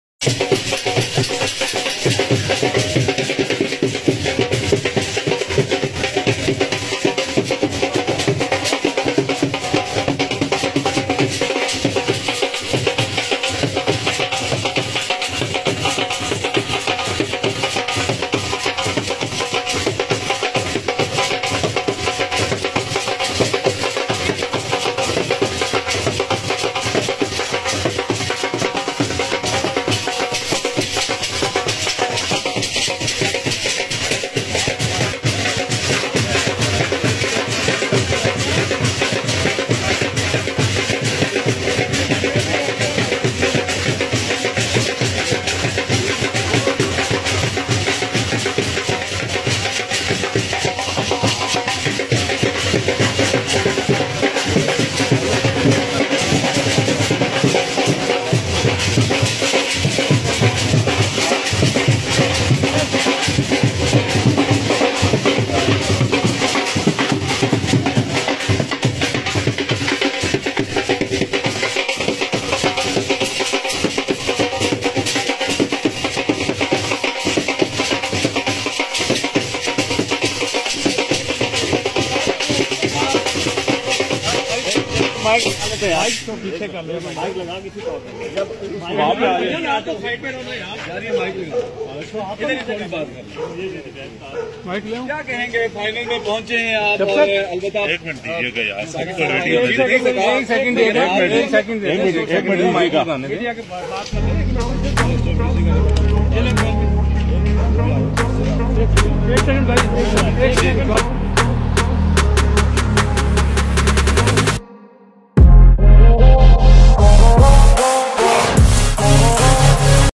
Haris Rauf Welcome Back to Pakistan After T20 Cricket World Cup
Haris Rauf Welcome Back to Pakistan After T20 Cricket World Cup Australia 2022
Haris Rauf media talk outside his home in islamabad
Haris Rauf Returned to Pakistan and received a great welcome by Locals in his Neighbour hood . Haris Rauf is a strike Bowler for Pakistan International Cricket Team
According to Pakistan's top pacer Haris Rauf, the entire nation is pleased with the national team that made it to the T20 World Cup final and finished as runners-up in the major competition.

The bowler, who put up some outstanding performances in the just finished T20 World Cup 2022, was warmly welcomed home to Pakistan by friends and family in Islamabad.
At his reception outside his house, fans danced to the beat of a Dhol.

Speaking to the media, Haris thanked his fans for their support.

"You love us so much and this is the reason why we try to perform well," he said.

"Alhamdulillah, we are all happy with the World Cup and I think all of Pakistan is happy with our performance,"
Haris is looking forward for upcoming home test series against england,
England cricket team is playing test match series after 22 years.